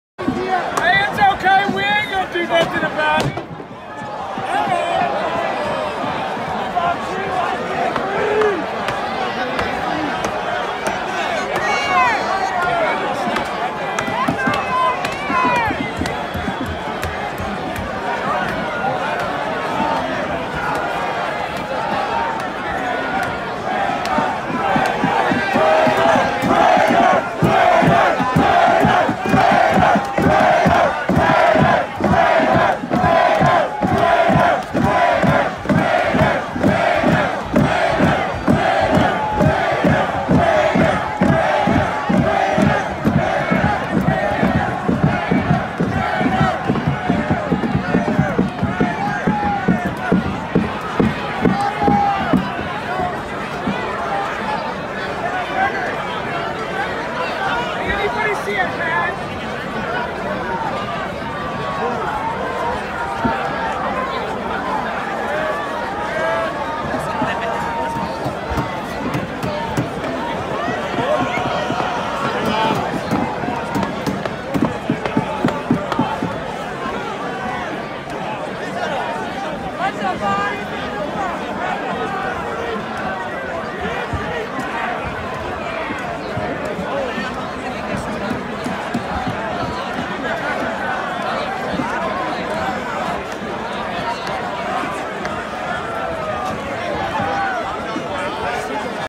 Washington DC Riots Turn Violent As Protestors Resort To Breaking Windows
Washington DC riots begin to escalate as protestors resort to breaking windows on The US Capitol Building.